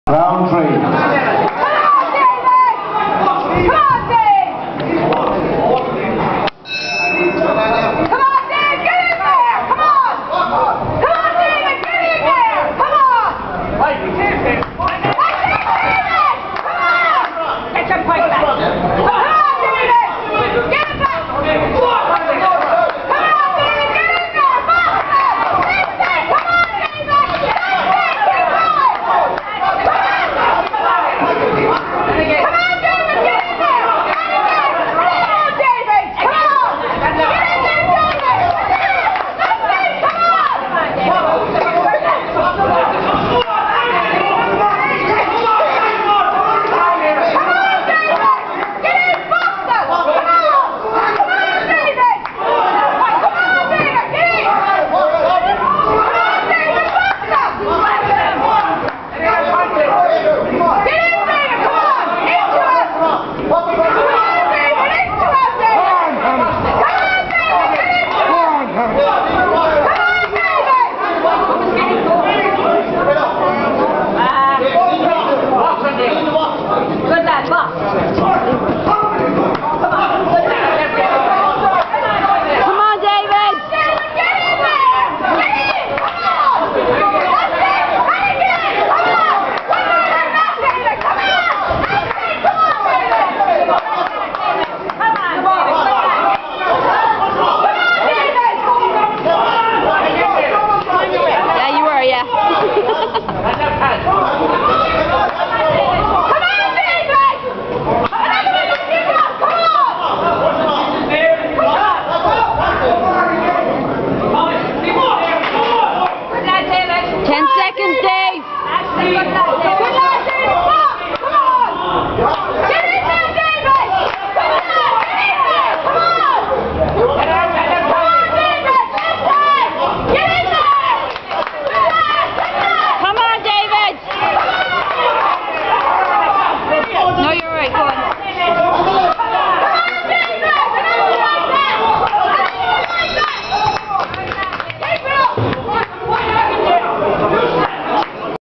Boxing Under-19 semi final
David Barrett Geesala Boxing Club, semi-final 75kg in National Stadium